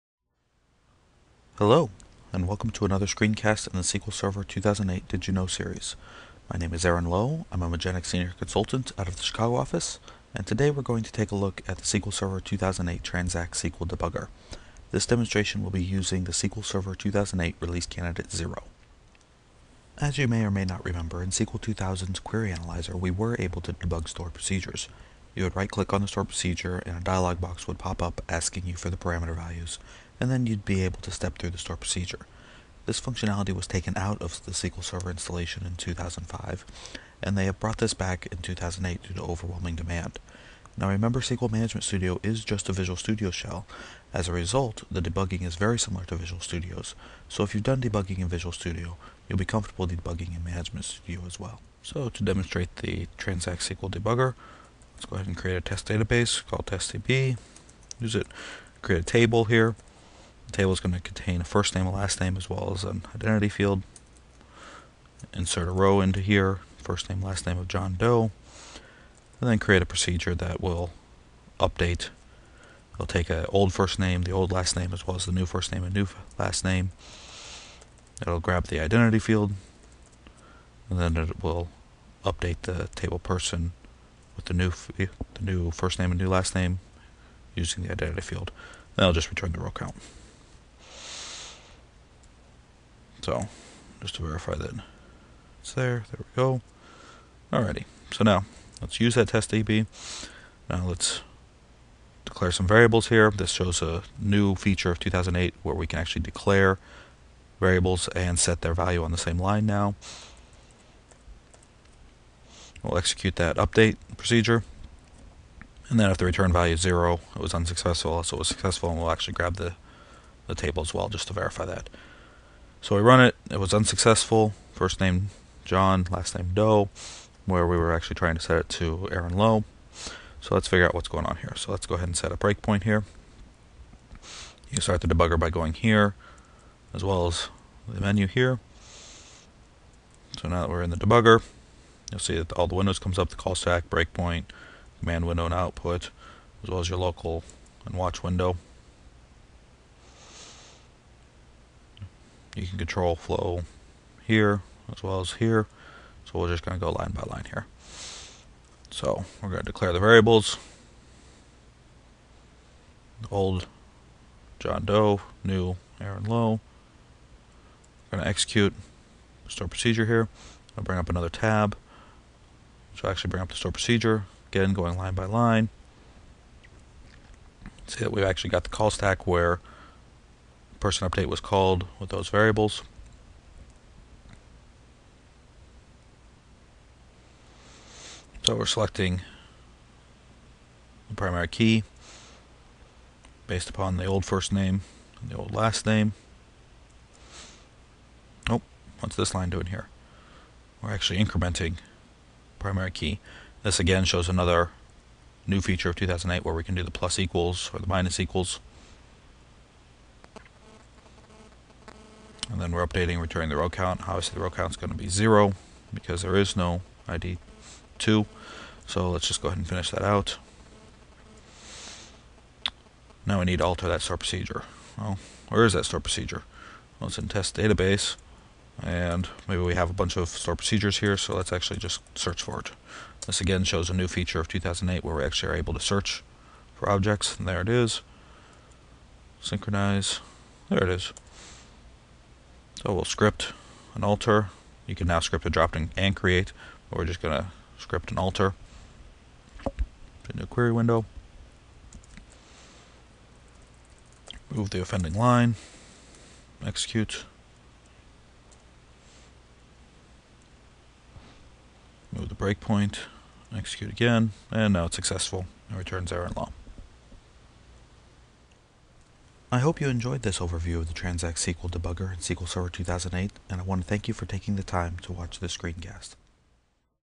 SQL Server 2008 T-SQL Debugger
The Transact-SQL debugger in SQL Server Management Studio enables you to find errors in Transact-SQL scripts, stored procedures, triggers, and functions by observing their run-time behavior. You can start the debugger when you are using the Database Engine Query Editor window.

By using the Transact-SQL debugger, you can do the following:

• Step through the Transact-SQL statements in the editor line by line, or set breakpoints to stop at specific lines.
• Step into or over Transact-SQL stored procedures, functions, or triggers that are run by the code in the editor window.
• Watch the values that are assigned to variables, and observe system objects such as the call stack and threads.